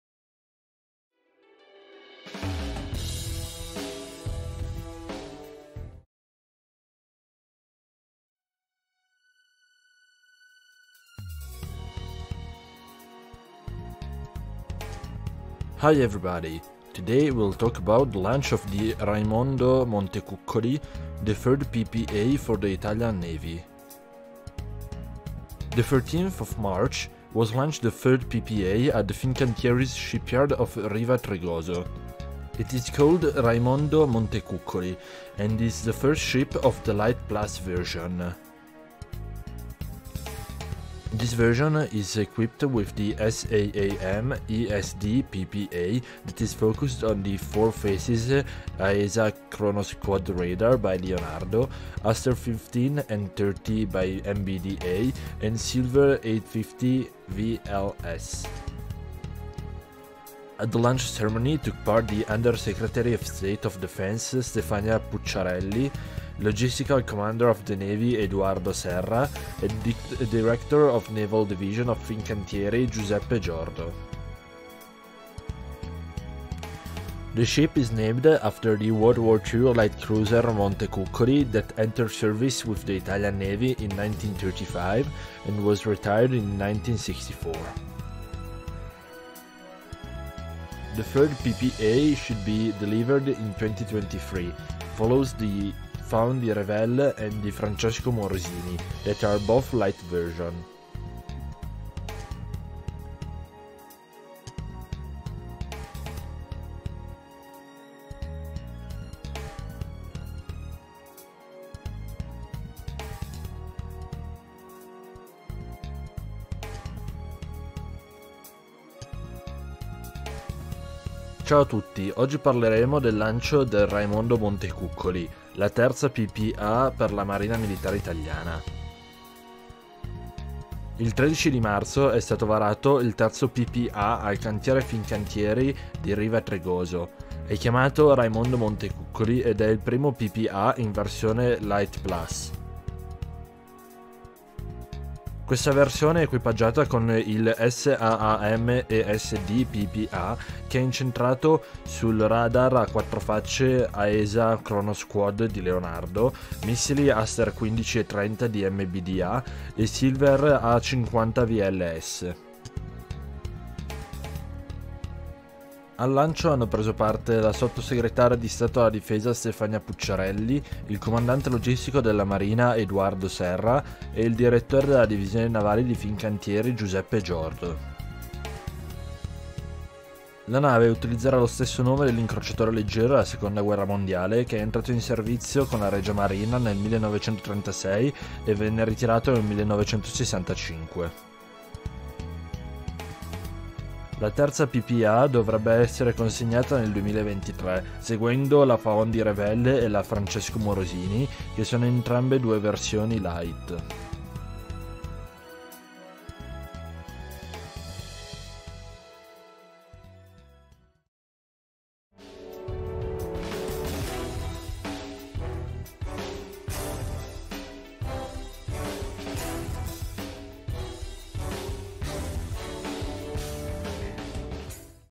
Launched the 3rd Italian PPA - Raimondo Montecuccoli
Italian section starts at 2:00
La sezione Italiana comincia a 2:00

Hi everybody,
Today we will talk about the launch of the Raimondo Montecuccoli, the third PPA for the Italian Navy